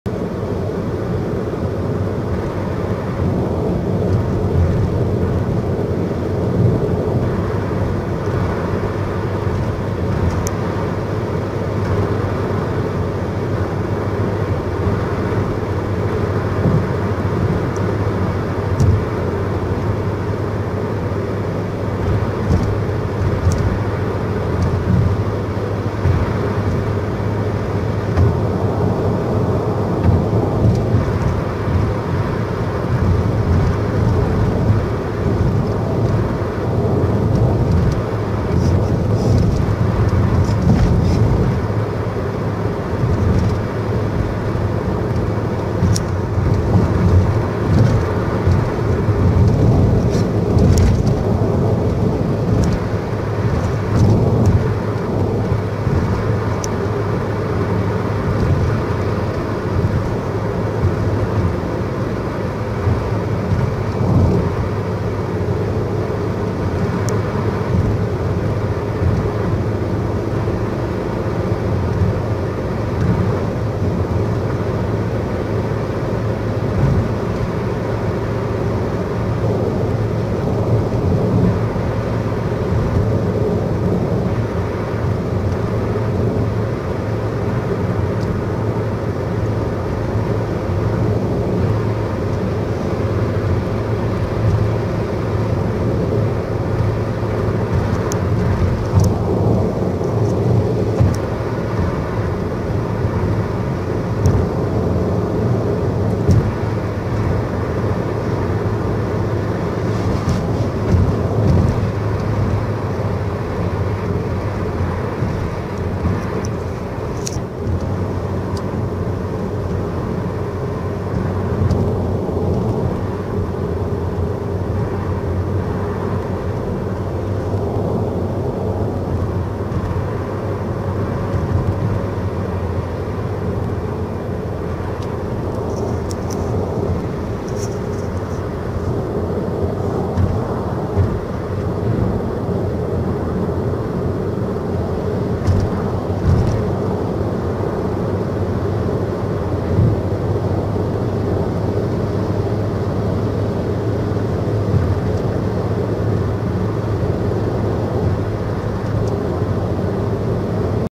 Así está la A-7 a su paso por el sur de la provincia de Castellón
En el vídeo adjunto a esta noticia se puede comprobar el mal estado de la carretera A-7 a su paso por el sur de la provincia de Castellón, más concretamente tras pasar la Vall de Uxó yendo en dirección sur.